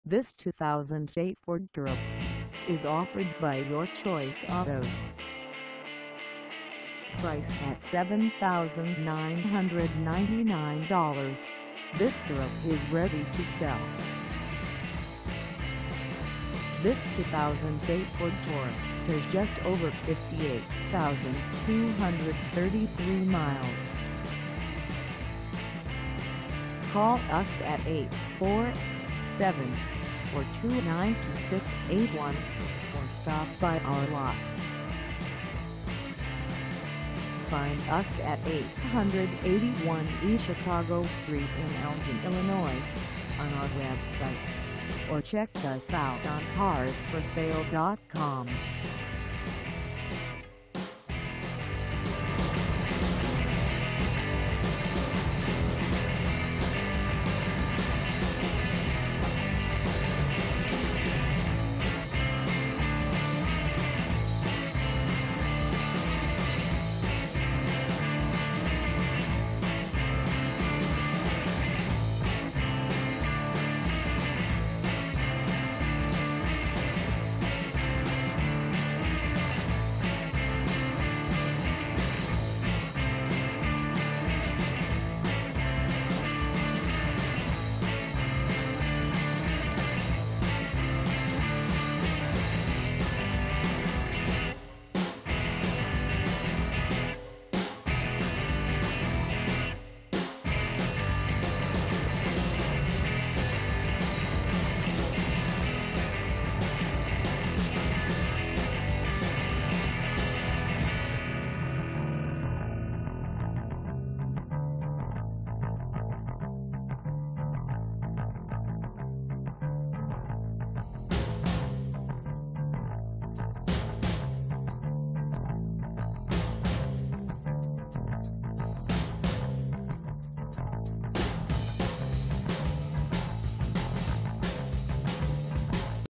2008 Ford Taurus SEL 4dr Sedan for sale in Elgin, IL 60120 a
This 2008 Ford Taurus SEL 4dr Sedan is for sale in Elgin, IL 60120 at Elgin.

Engine: 3.5L V6
Transmission: Automatic 6-Speed
Color: Silver
Mileage: 58233
Contact Your Choice Autos 881 E. Chicago St. Elgin, IL 60120 to test drive this 2008 Ford Taurus SEL 4dr Sedan today.